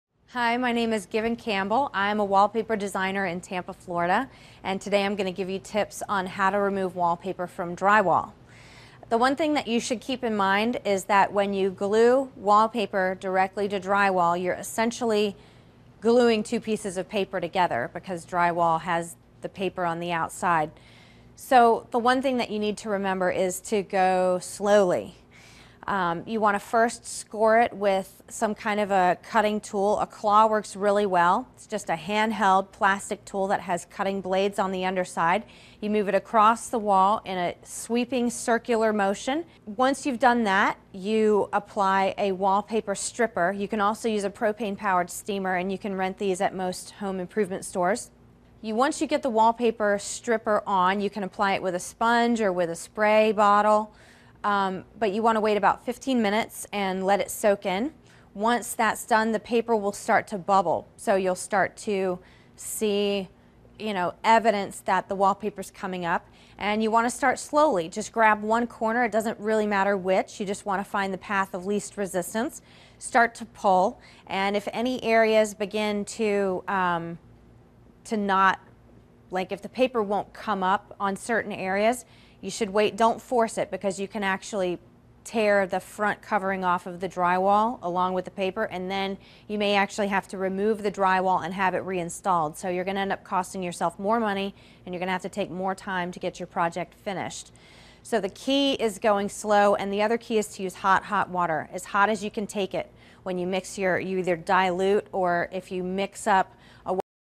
How to Remove Wallpaper From Drywall
How to Remove Wallpaper From Drywall. Part of the series: Removing Wallpaper. Wallpaper can be removed from drywall in a very special way. Remove wallpaper from drywall with help from a custom wallpaper designer in this free video clip.